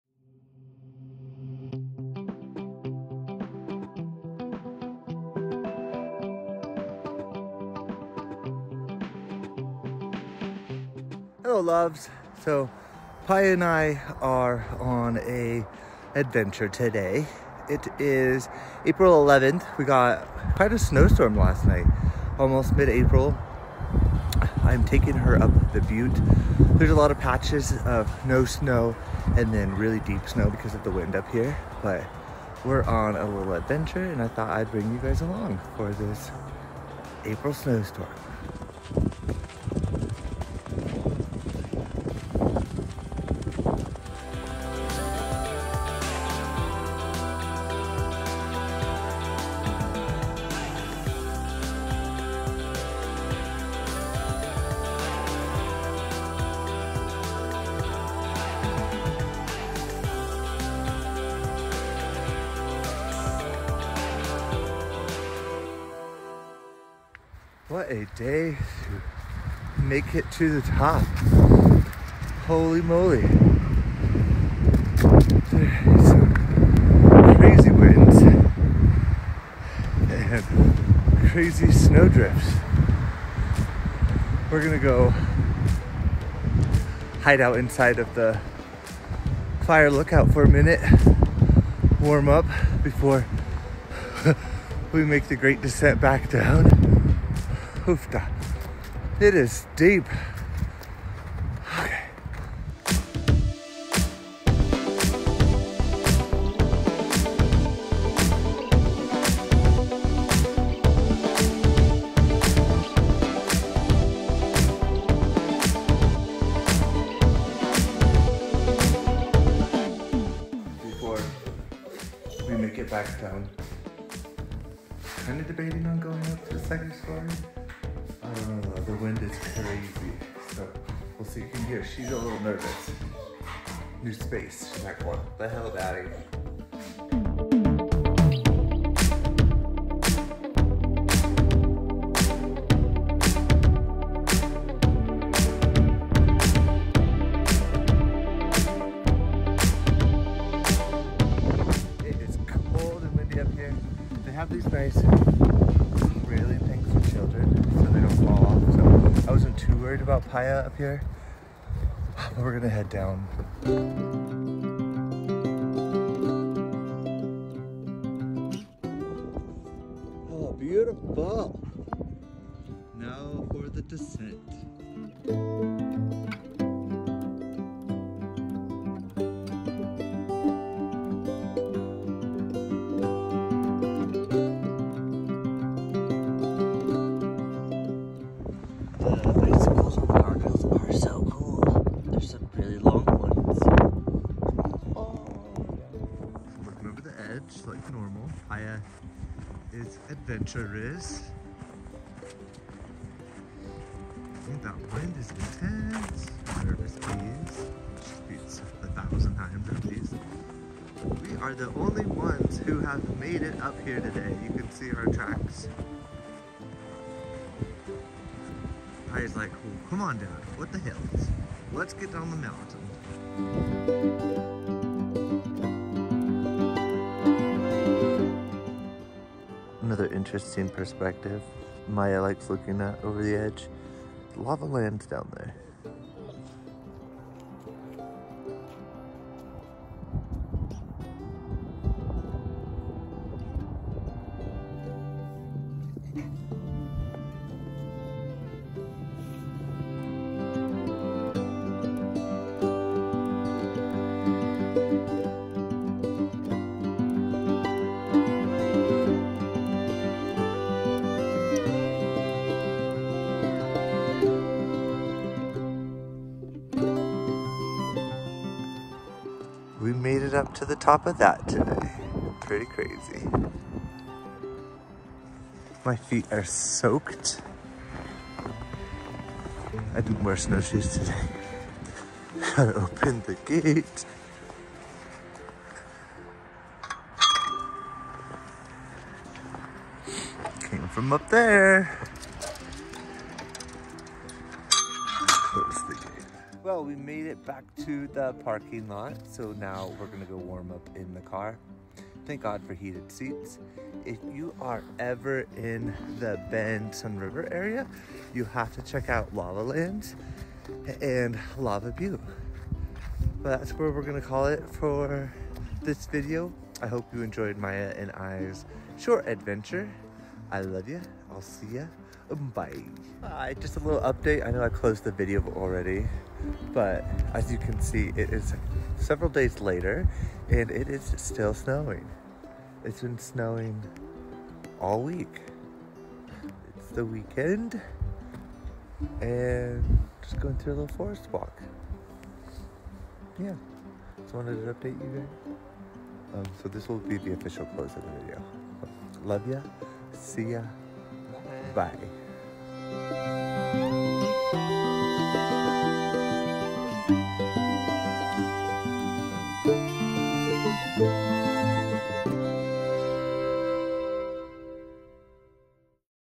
April Snowstorms II Lava Butte II Bend Oregon
Welcome Back!

In this weeks video Maya and I hiked up Lava Butte in an unexpected April Snowstorm. This snow seemed to not want to go away and it has still been snowing for over a week now. It's like winter in the middle of spring. Not too unusual for the Bend Oregon area, but out of this years norm/ pattern for sure. I hope you enjoy the vlog.

Loves ya all!